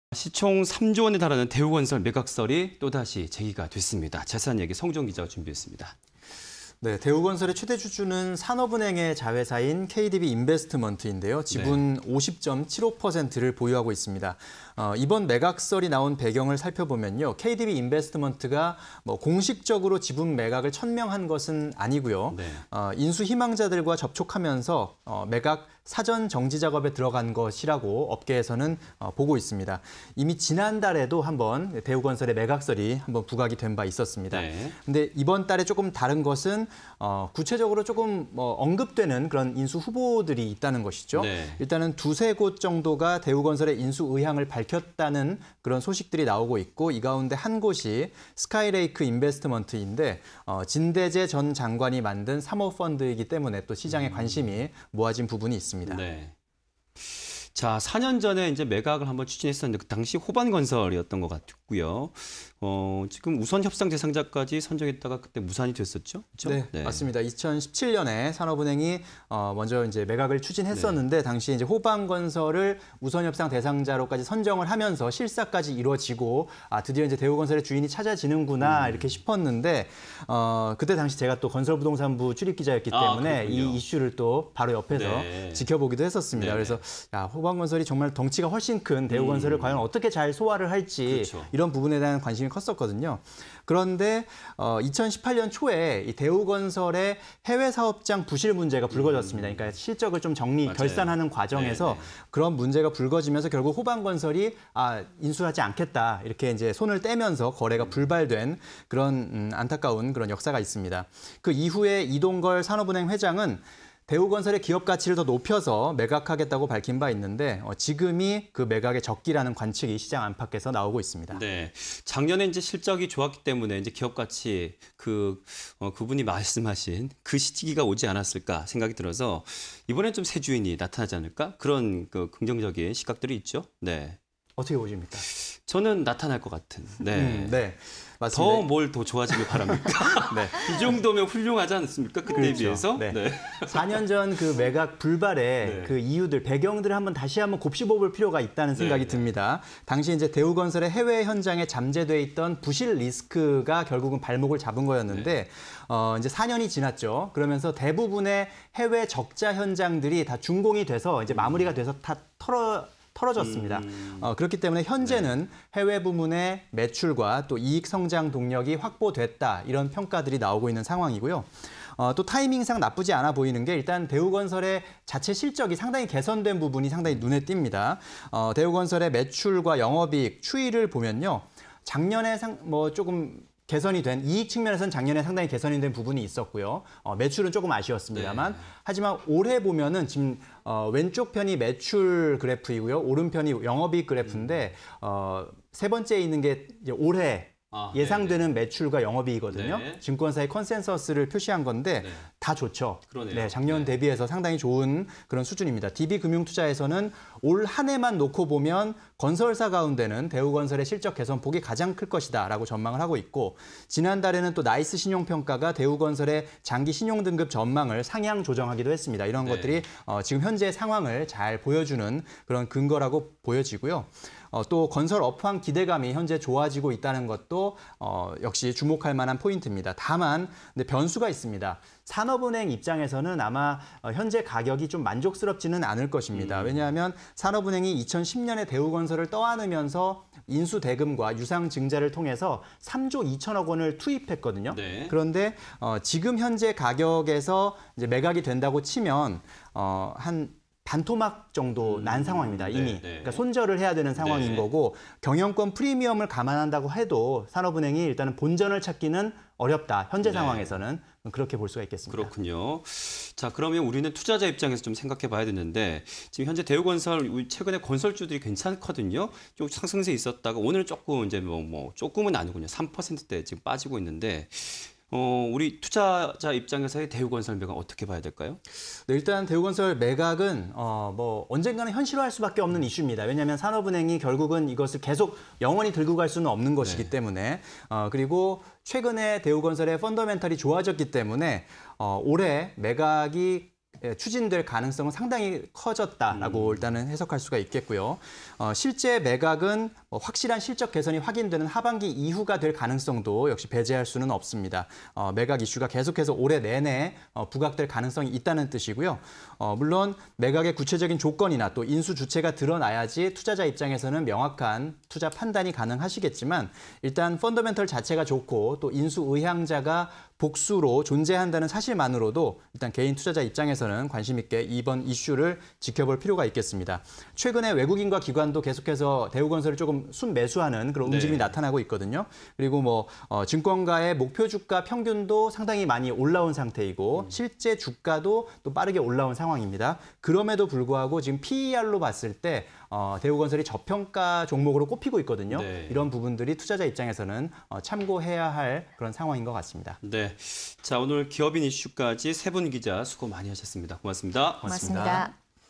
또다시 불거진 대우건설 매각설… 새주인 찾나?_기업IN이슈 (20210421)
- 진대제 전 장관의 스카이레이크 등 2~3곳 인수 관심
- 해외사업장 부실 논란에 불발… “올해가 매각 적기”
- 해외 적자 현장 대부분 마무리… 성장 동력 확보
- 매각 이슈 올해 내내 부각 가능성… 펀더멘탈 주시